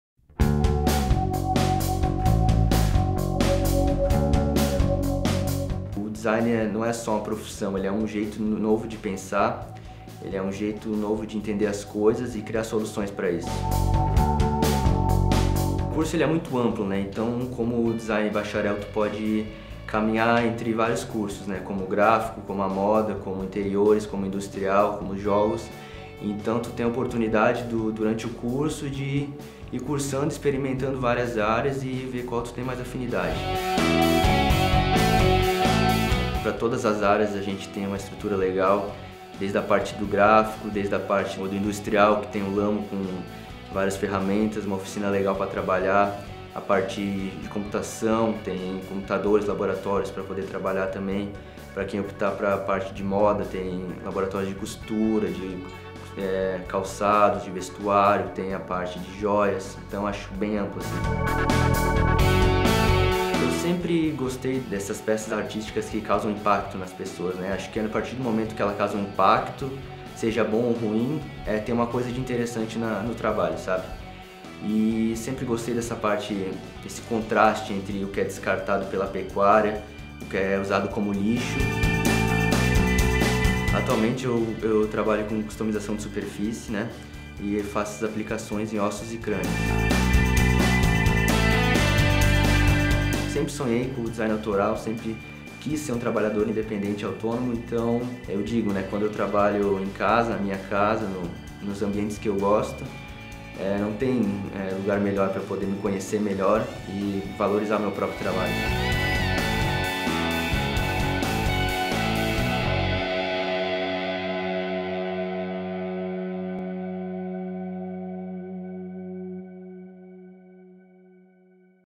QUERO ESTUDAR – Design Univali
Para Manoel, Design não é apenas uma profissão, mas um jeito novo de pensar, de entender as coisas e criar soluções. Foi experimentando várias áreas dentro do design, que ele descobriu o seu talento para customização de superfícies, que ele aplica em ossos e crânios.

DESIGN
Título: Bacharel
Modalidade: Presencial
Duração: 8 semestres (4 anos)
Campi: Balneário Camboriú